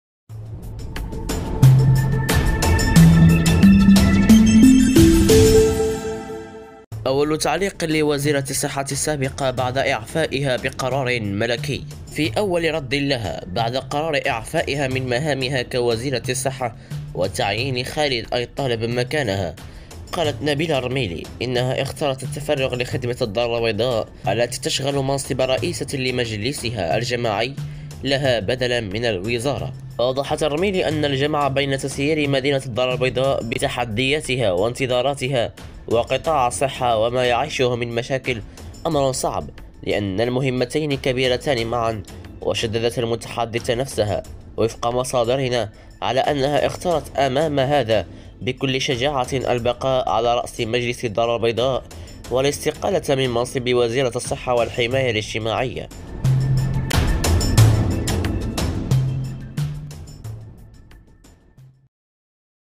عاجل.. أول تعليق لوزيرة الصحة السابقة بعد اعفاءها بقرار ملكي
عاجل.. أول تعليق لوزيرة الصحة السابقة بعد
اعفاءها بقرار ملكي

في
أول رد لها بعد قرار إعفائها من مهامها كوزيرة الصجة
وتعيين خالد آیت الطالب مكانها، قالت نبيلة الرميلي، إنها
اختارت التفرغ لخدمة الدار البيضاء التي تشغل منصب
رئيسة لمجلسها الجماعي لها بدلا من الوزارة.